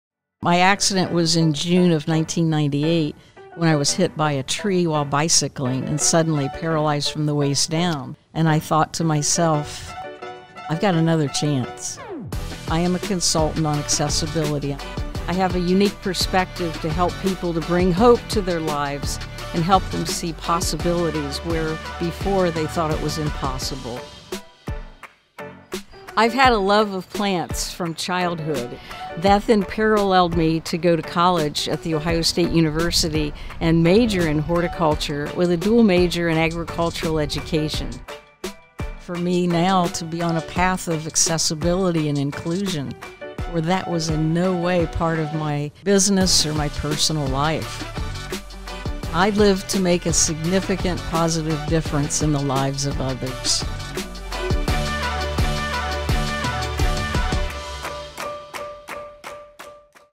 Rosemarie Rossetti, Ph.D., PCMA CL23 Trailblazer
Rosemarie Rossetti, Ph.D., internationally recognized expert on disability inclusion and accessibility, has built a legacy by inspiring people around the world. For more information on Dr. Rossetti or to register for Convening Leaders 2023,